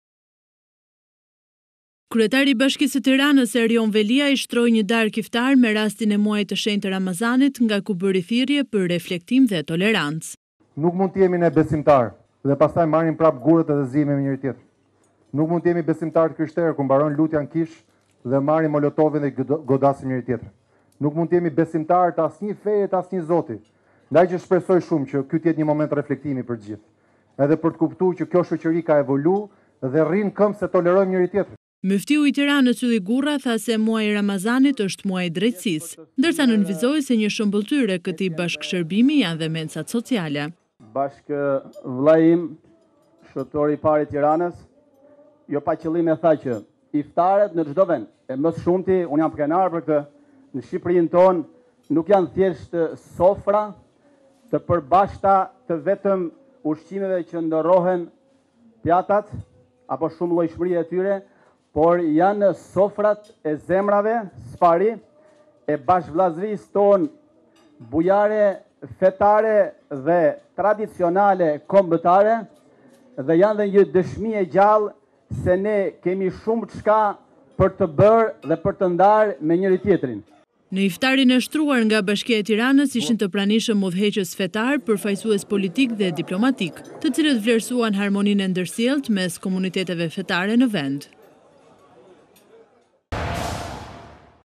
RTV Ora - Erion Veliaj shtron iftar
Kryetari i Bashkisë së Tiranës, Erion Veliaj shtroi   një darkë Iftari Me rastin e Muajit të shenjtë të Ramazanit, nga ku bëri thirrje dhe për reflektim dhe tolerancë.

Veliaj: Nuk mund të jemi besimtarë kur respektojmë namazin dhe pastaj marrim prapë gurët dhe zihemi me njëri-tjetrin; nuk mund të jemi besimtarë të krishterë kur mbaron lutja në kishë dhe marrim topin e godasim njëri- tjetrin; kështu, s’mund të jemi besimtarë të asnjë feje, e të asnjë Zoti. Ndaj, shpresoj shumë që ky të jetë një moment reflektimi për të gjithë, edhe për të kuptuar që kjo shoqëri ka evoluar dhe rri në këmbë, sepse tolerojmë njëri-tjetrin”, shtoi kryebashkiaku.

Myftiu i Tiranës, Ylli Gurra tha se muaji i Ramazanit është muaji i drejtësisë dhe nënvizoi se një shëmbëlltyrë e këtij bashkëshërbimi janë edhe mensat sociale.